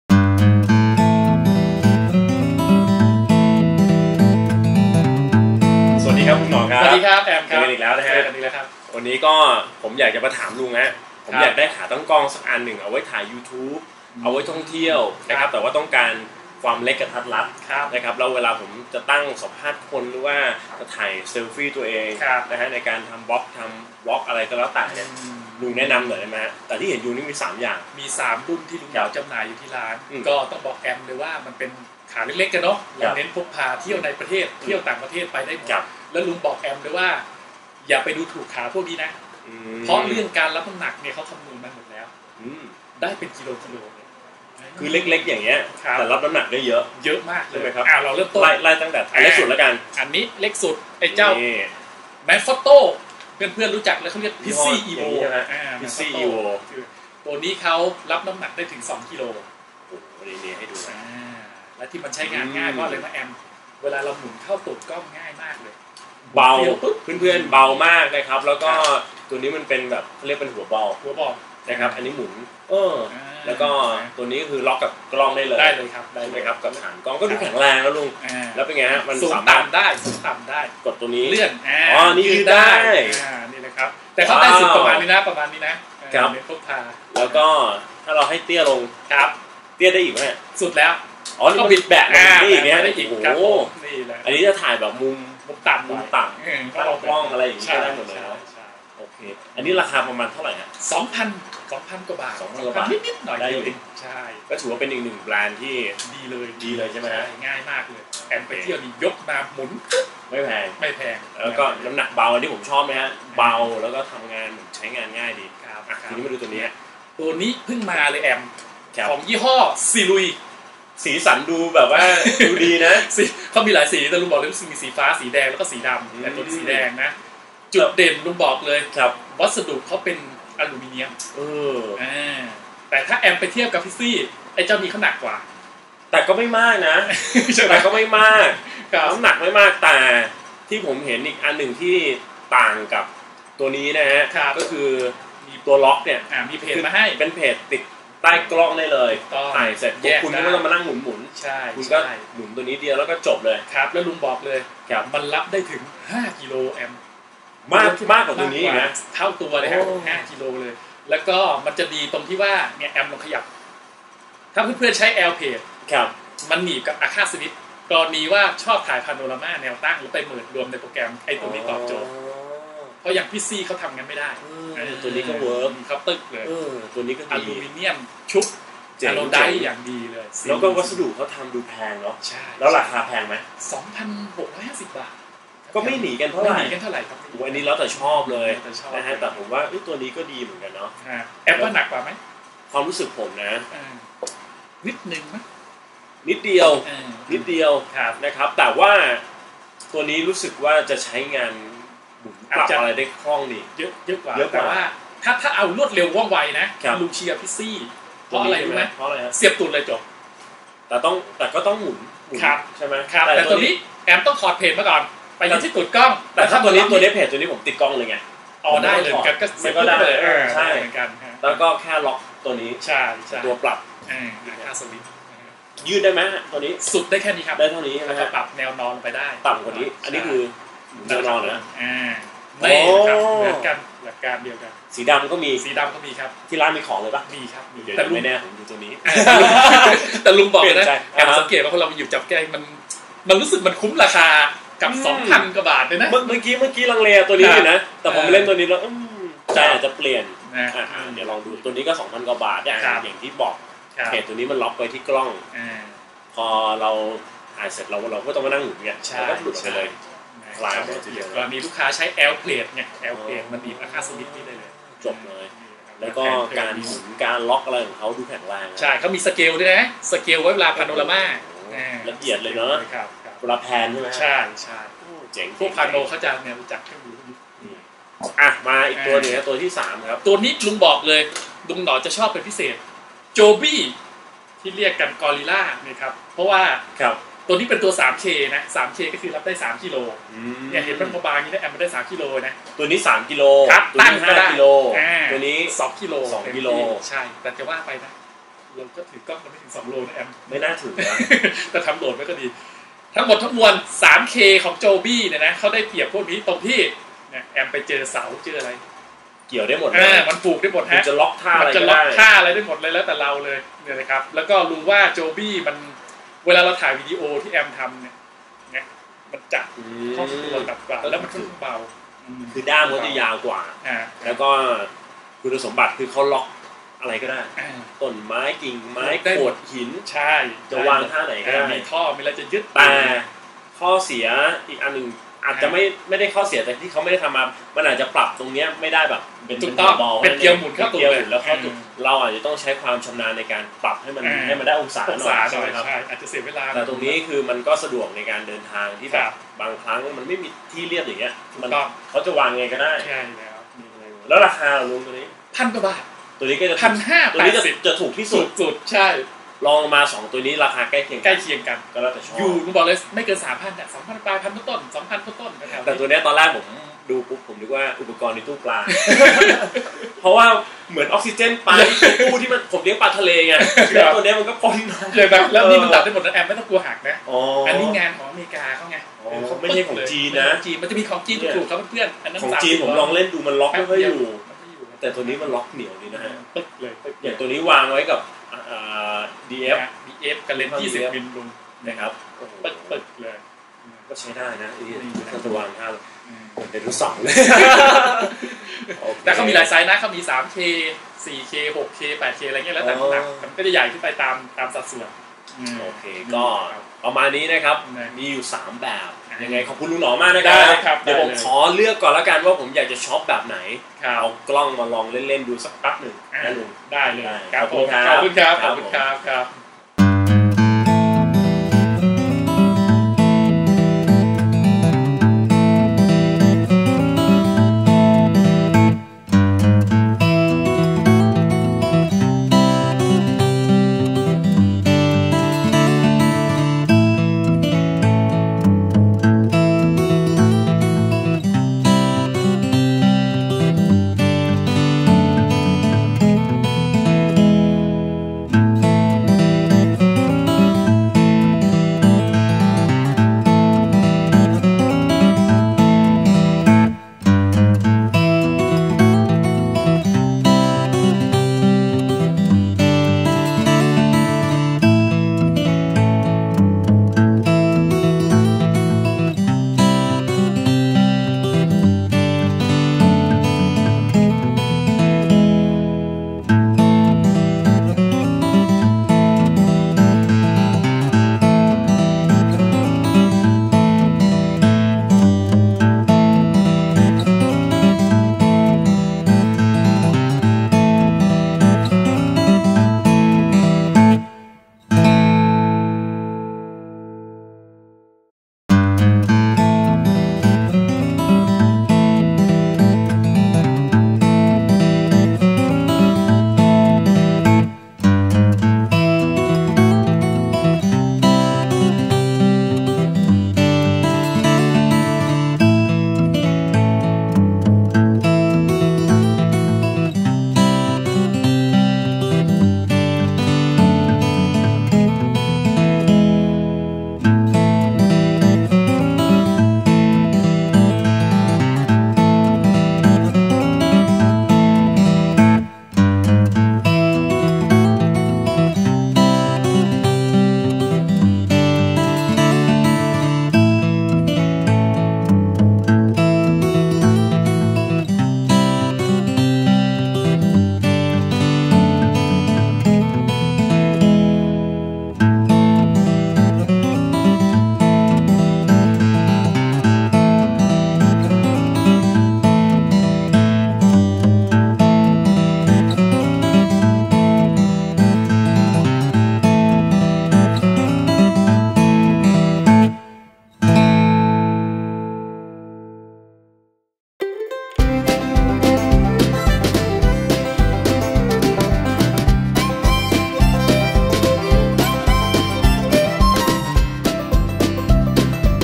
รีวิวขาตั้งกล้องขนาดเล็ก พกพาง่าย by nhor shop | ร้านลุงหนอ
รีวิวขาตั้งกล้องขนาดเล็ก พกพาง่าย สนใจแวะมาลองได้ที่ร้านลุงหนอนะครับ
Nhor Shop